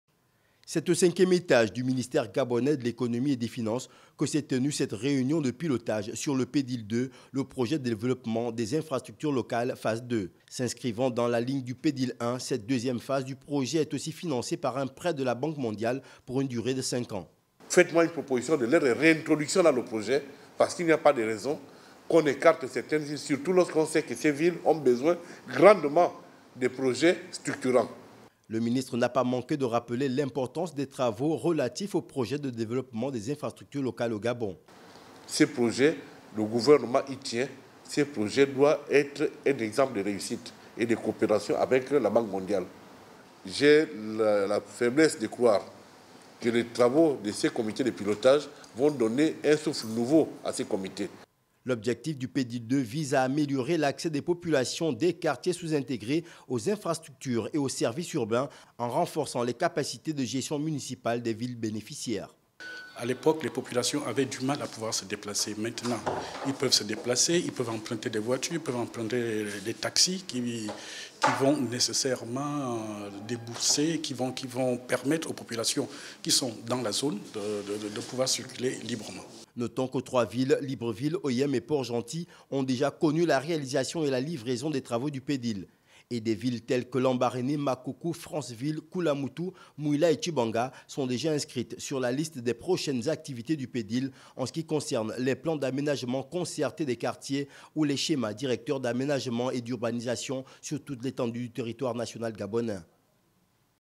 GABON - INFRASTRUCTURE ROUTIÈRE RÉUNION DU COMITÉ DE PILOTAGE PDIL
Ce vendredi 3 Janvier, s’est tenue, au Ministère de l'Économie et des Finances, une réunion de pilotage sur le projet de Développement des Infrastructures Locales, dans sa deuxième.